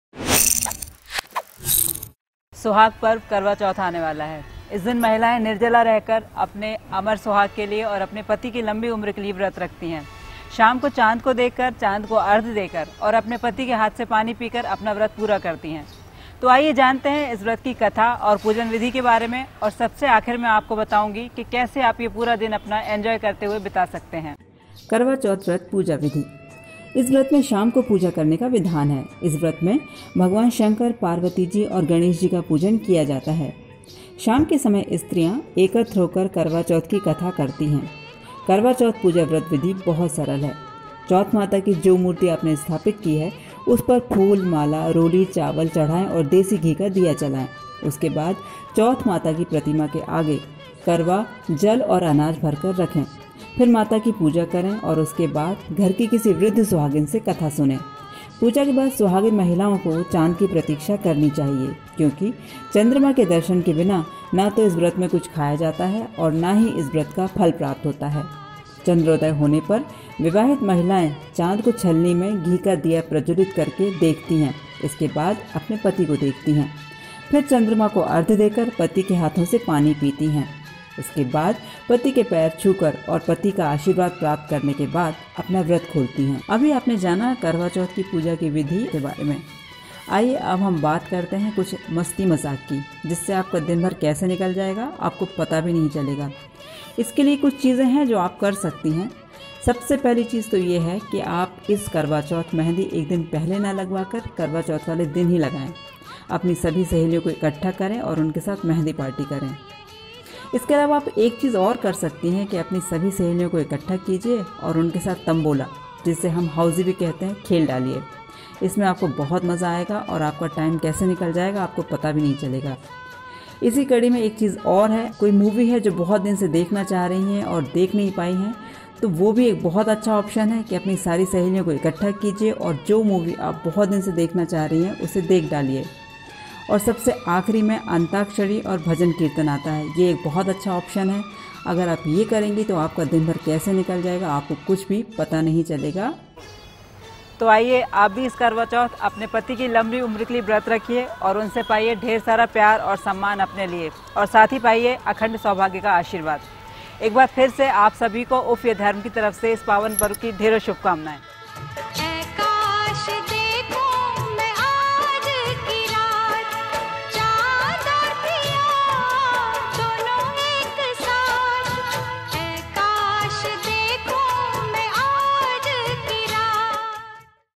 सुहागनें ज़रूर देखें
करवाचौथ पर खास । कैसे रखें व्रत,क्यों रखा जाता है व्रत और इस दिन क्या करें खास